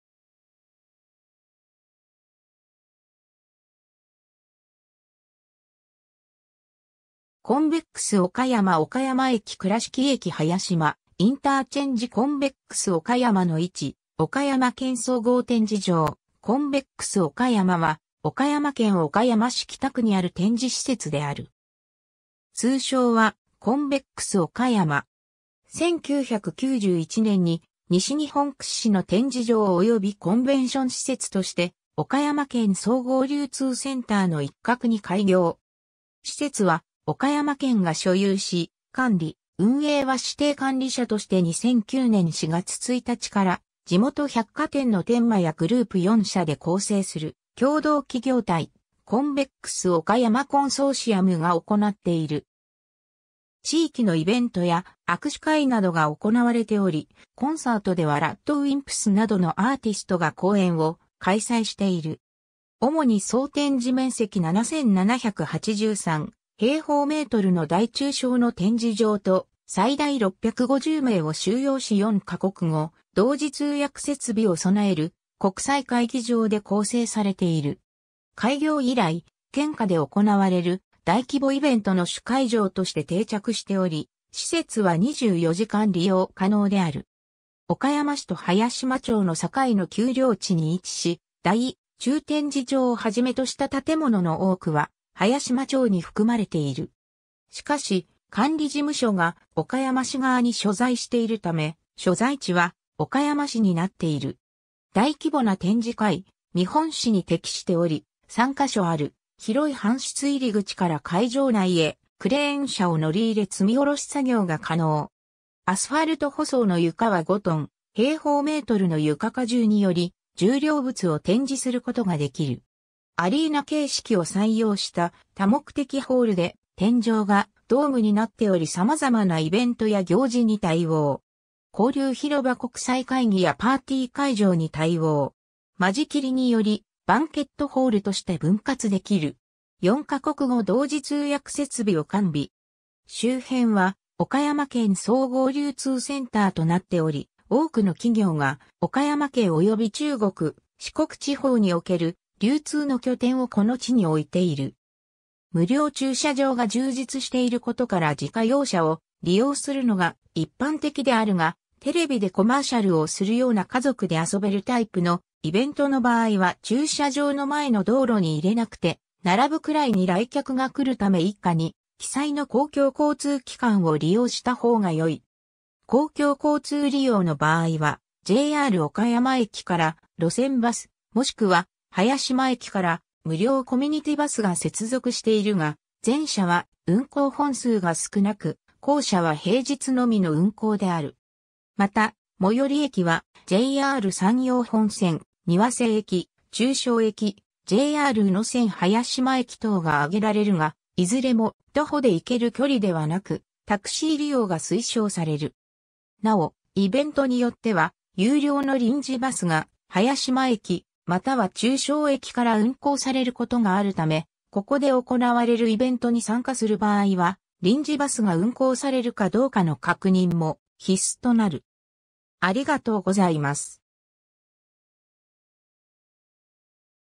岡山県総合展示場コンベックス岡山
コンベックス岡山 岡山駅 倉敷駅 早島IC コンベックス岡山の位置（岡山県） 岡山県総合展示場コンベックス岡山（おかやまけんそうごうてんじじょうコンベックスおかやま、Convex Okayama）は岡山県岡山市北区にある展示施設である。
通称は「コンベックス岡山」。
 1991年に西日本屈指の展示場及びコンベンション施設として岡山県総合流通センターの一画に開業。
施設は岡山県が所有し、管理・運営は指定管理者として2009年4月1日から地元百貨店の天満屋グループ4社（丸田産業、ハウジング山陽、山陽セフティ、天満屋アドセンター）で構成する共同企業体「コンベックス岡山コンソーシアム」が行っている。
 地域のイベントや握手会などが行われており、コンサートではRADWIMPSなどのアーティストが公演を開催している。
 主に総展示面積7,783m2の大中小の展示場と、最大650名を収容し4か国語同時通訳設備を備える国際会議場で構成されている。
開業以来、県下で行われる大規模イベントの主会場として定着しており、施設は24時間利用可能である。
 岡山市と早島町の境の丘陵地に位置し、大・中展示場をはじめとした建物の多くは早島町に含まれている。
しかし、管理事務所が岡山市側に所在しているため、所在地は岡山市になっている。
 大規模な展示会･見本市に適しており、3箇所ある広い搬出入口から会場内へクレーン車を乗り入れ積み下ろし作業が可能。
アスファルト舗装の床は5t/m2の床荷重により重量物を展示することができる。
 アリーナ形式を採用した多目的ホールで、天井がドームになっており様々なイベントや行事に対応。
 交流広場 国際会議やパーティー会場に対応。
間仕切りによりバンケットホールとして分割できる。
4ヶ国語同時通訳設備を完備。
 周辺は岡山県総合流通センターとなっており、多くの企業が岡山県及び中国・四国地方における流通の拠点をこの地に置いている。
 無料駐車場が充実していることから自家用車を利用するのが一般的であるが、テレビでコマーシャルをするような家族で遊べるタイプのイベントの場合は駐車場の前の道路に入れなくて並ぶくらいに来客が来るため以下に記載の公共交通機関を利用した方が良い。
 公共交通利用の場合はJR岡山駅から路線バス（イベント開催時の平日と毎週土・日曜と祝祭日のみ運行でそれ以外の日は、コンベックス岡山がある流通団地に入る手前のバス停が終点になる）、もしくは早島駅から無料コミュニティバスが接続しているが、前者は運行本数が少なく、後者は平日のみの運行である。
また、最寄り駅はJR山陽本線庭瀬駅（岡山市北区）、中庄駅（倉敷市）、JR宇野線早島駅（早島町）等が挙げられるが、いずれも徒歩で行ける距離ではなく、タクシー利用が推奨される。
 なお、イベントによっては有料の臨時バスが早島駅または中庄駅から運行される事があるため、（一部のイベントは岡山駅からも運行）ここで行われるイベントに参加する場合は、臨時バスが運行されるかどうかの確認も必須となる。
（毎年10月最初の土曜日と日曜日に行う岡山放送主催のおかやま元気まつりのように広告をたくさん出すイベントの場合はイベントのチラシやイベントのウェブサイトに臨時バスの事が時刻表付きで記載されていることがある。）